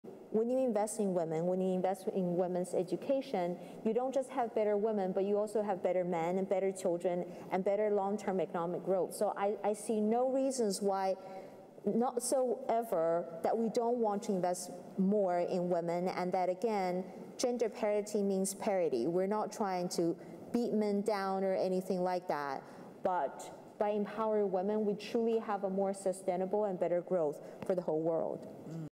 Liu Qian - Investing in Women for Sustainable Growth
Speaking during When Parity Meets Growth as part of the Annual Meeting of the New Champions 2024.